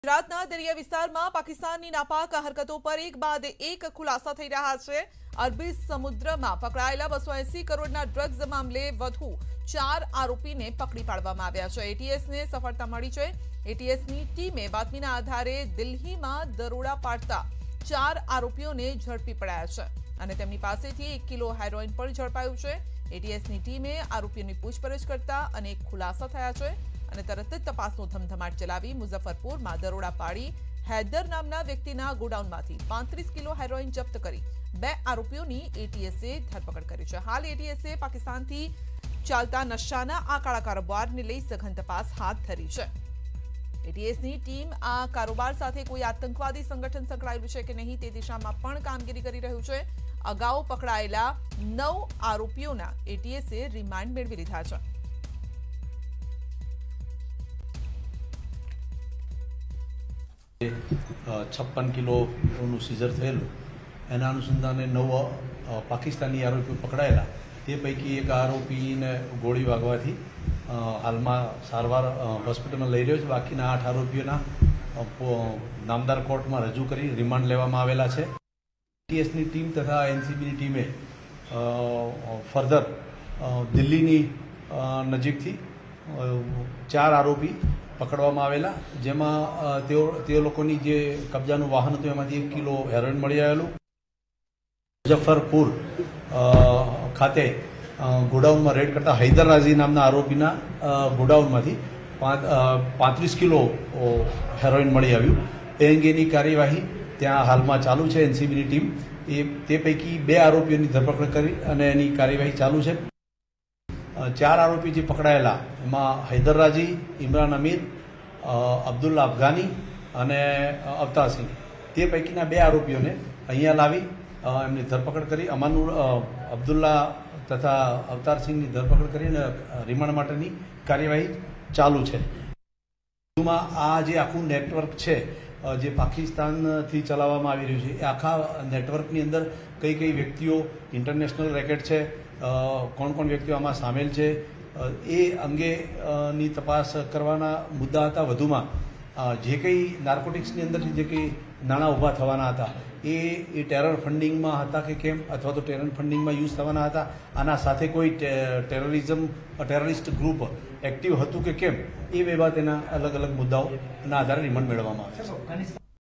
Gujarat Drug Bust | 280 કરોડના Drugs મામલે વધુ 4 લોકોની ધરપકડ | News18 Gujarati
280 કરોડના Drugs મામલે વધુ 4 લોકોની ધરપકડ. Delhi થી પકડાયેલા આરોપીઓ પાસેથી મળી આવ્યું Drugs. મુજફ્ફરપુરથી પકડાયેલા Haidar પાસેથી મળી આવ્યું 35 KG Heroine.